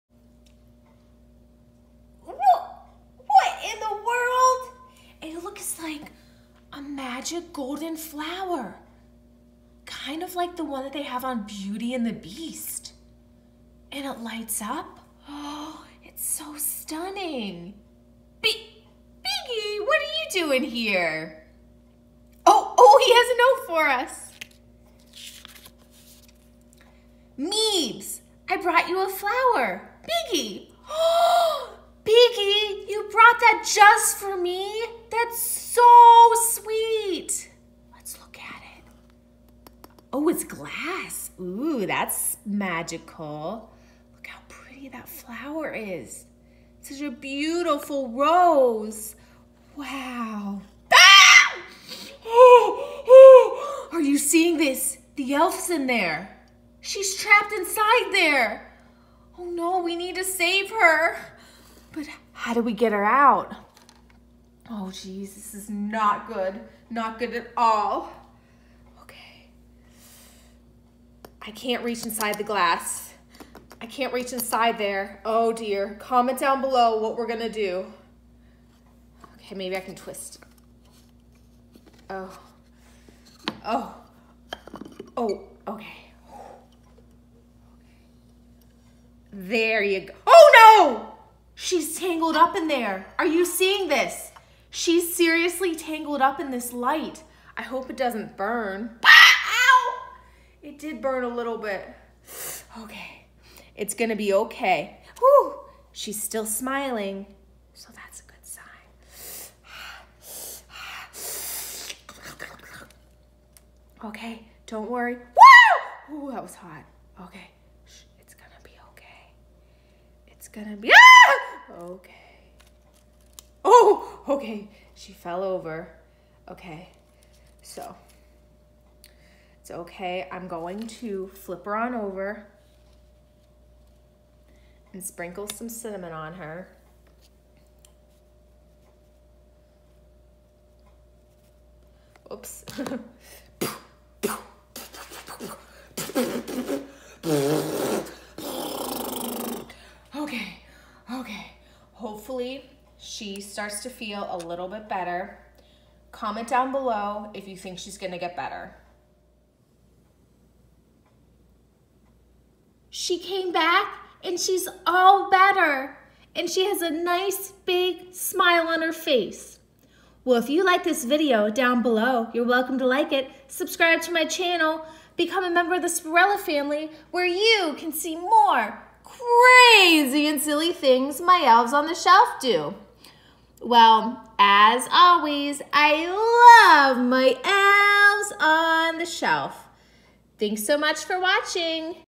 Elf TRAPPED inside ENCHANTED ROSE
Oh no Biggie did it again! He trapped on of the baby elves on the shelf inside the enchanted rose! How can we save her? Watch and find out what happens!